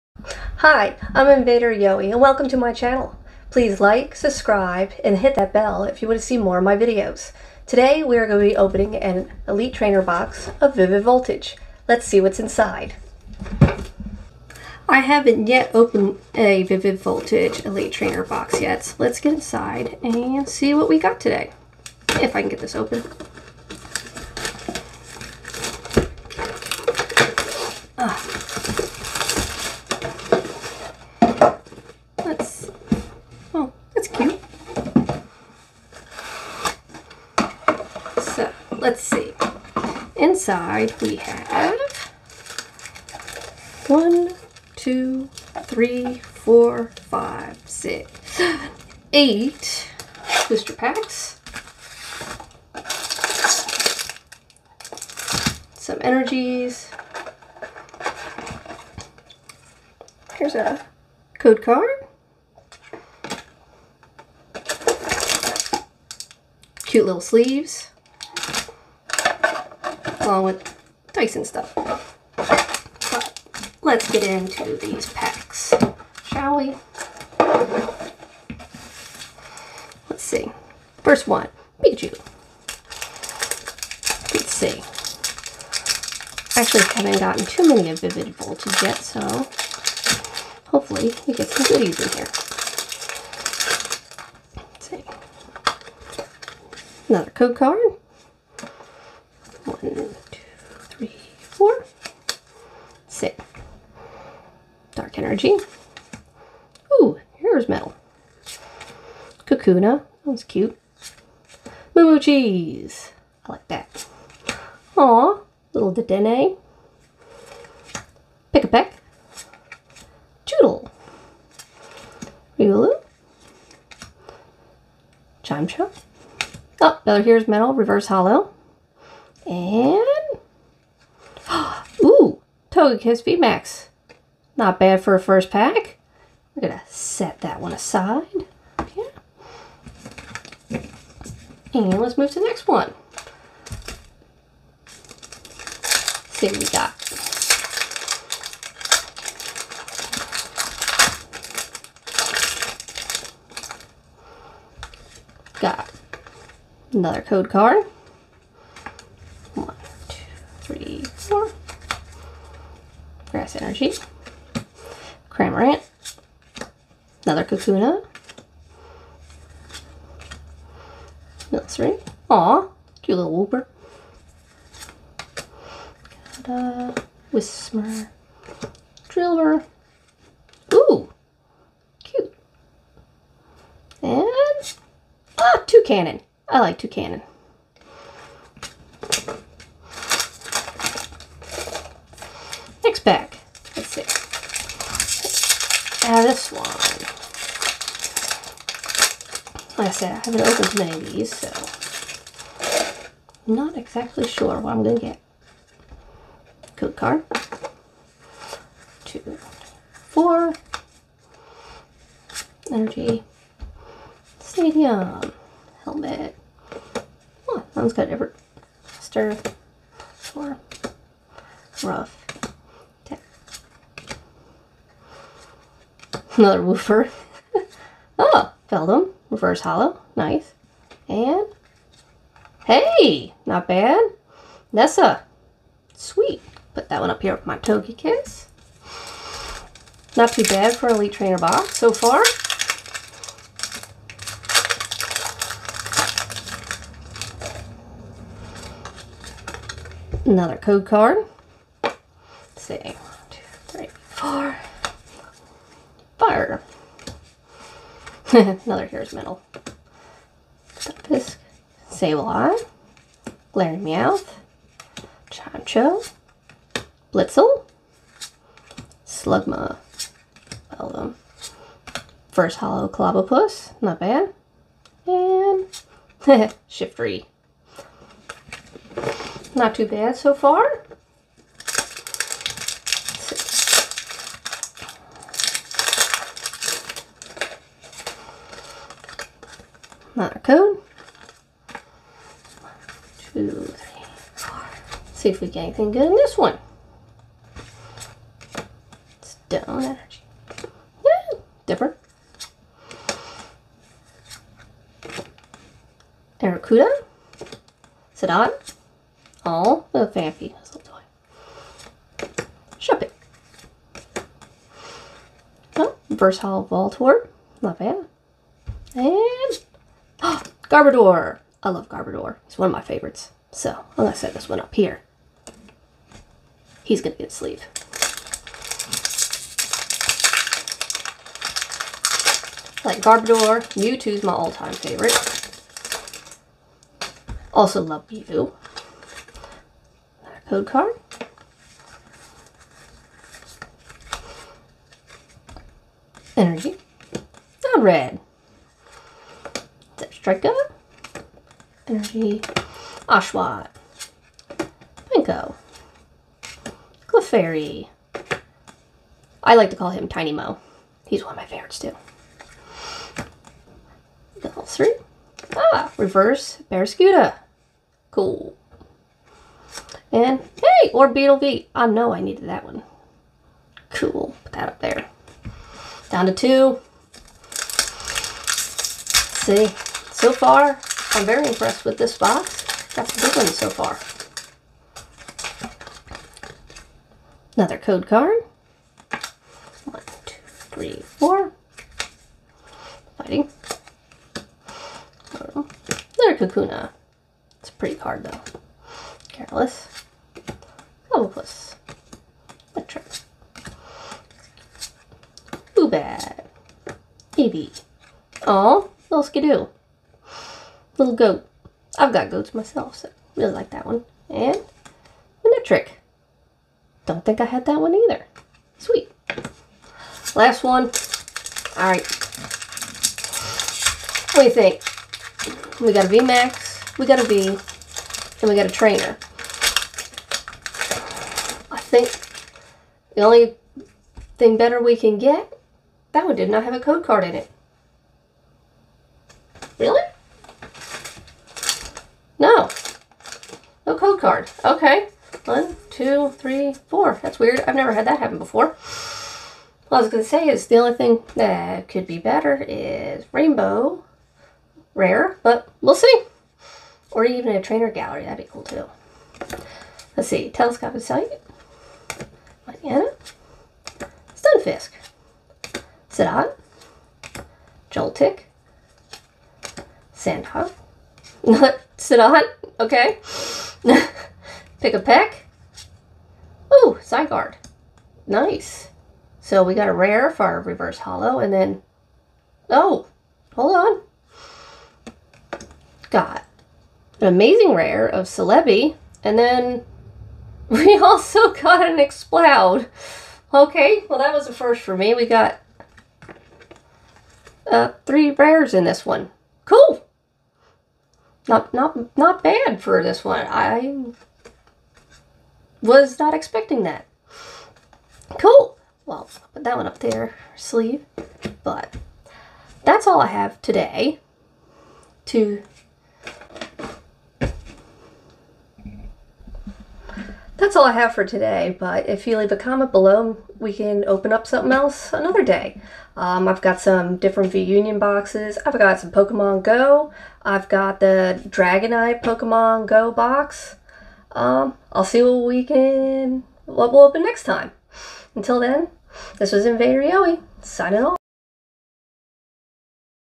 Vivid Voltage ETB with a Surprise Inside!
My very first video! Join my journey as I try to collect them all! I open a Vivid Voltage ETB and had a surprise inside that I had not encountered before.